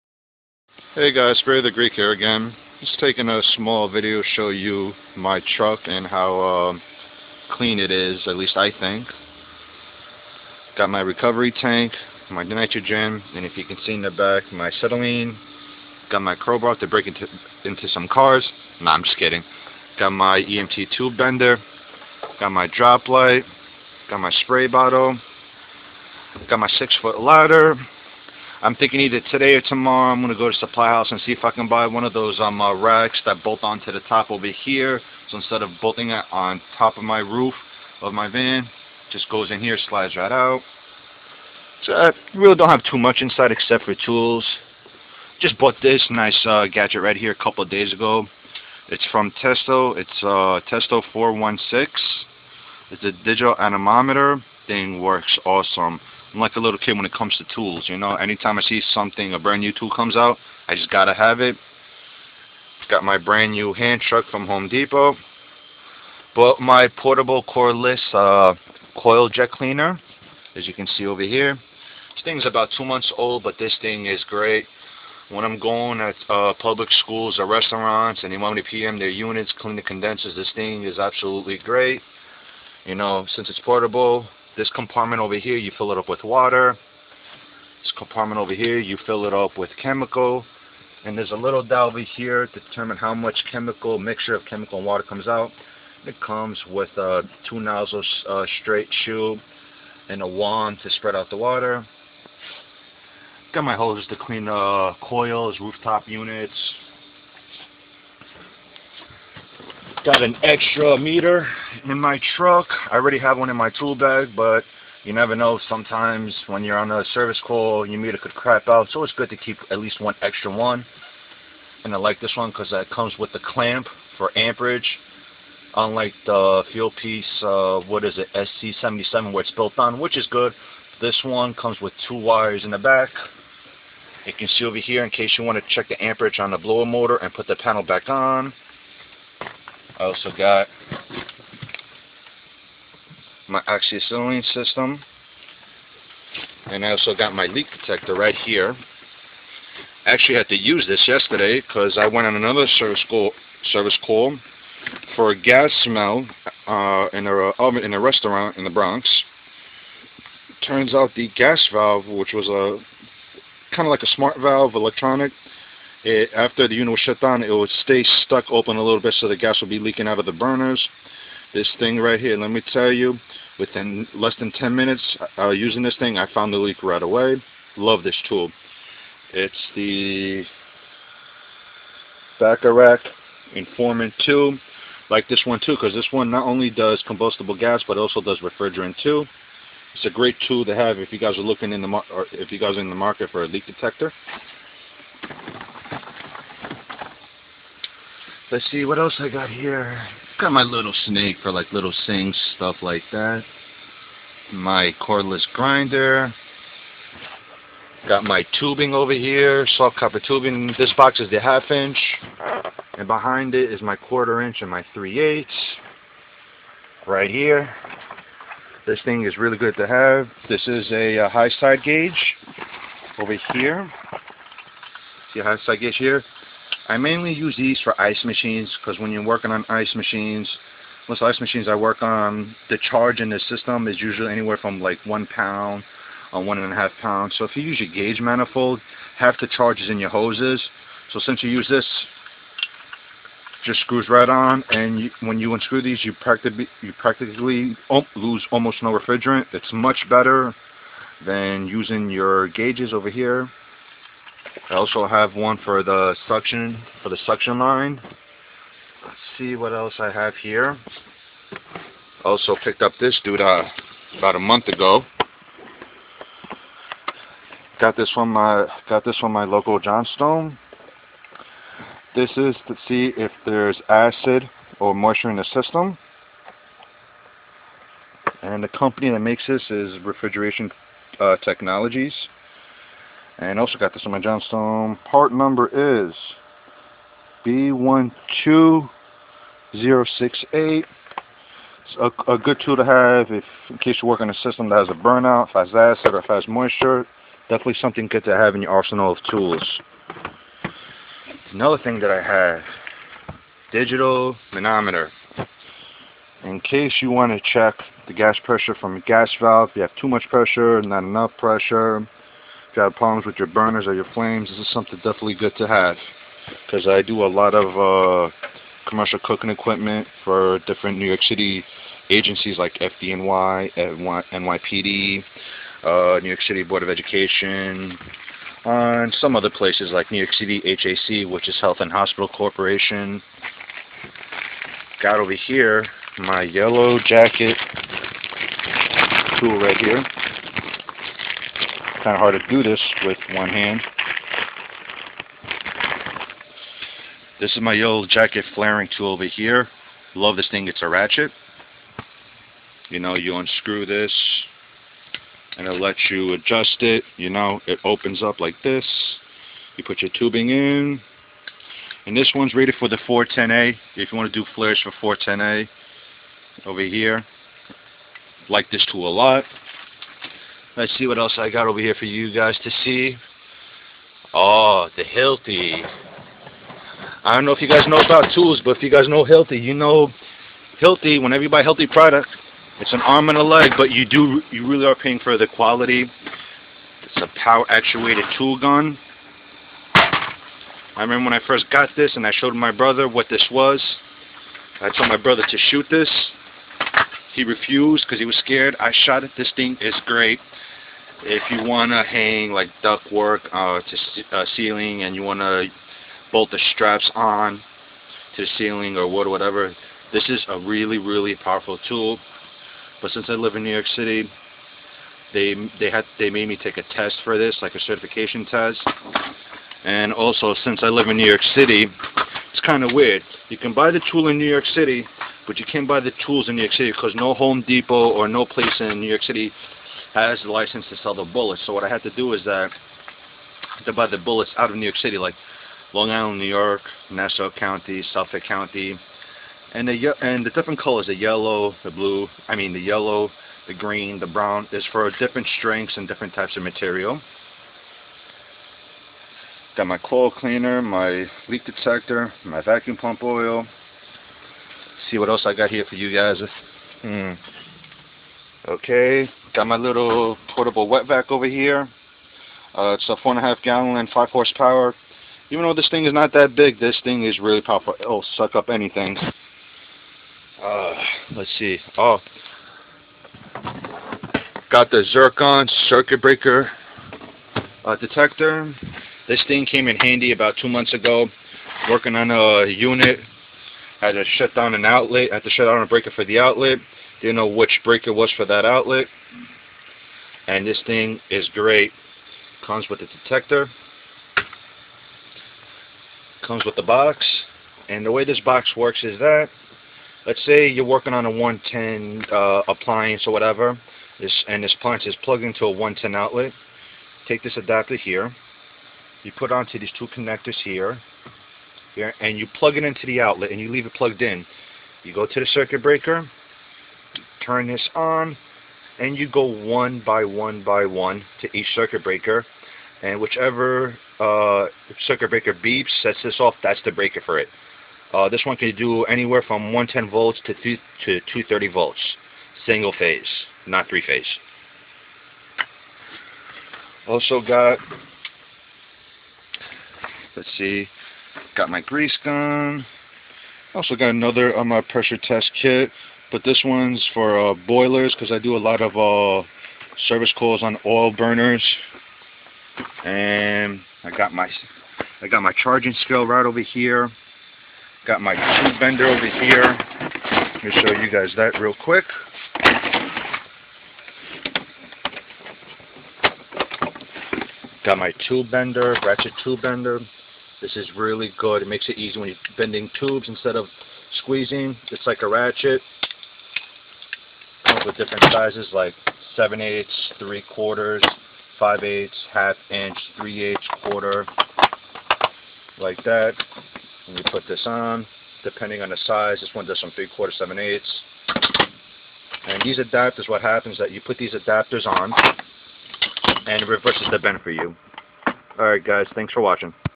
Hvac service van tools
Showing some of my toys that I have in my service van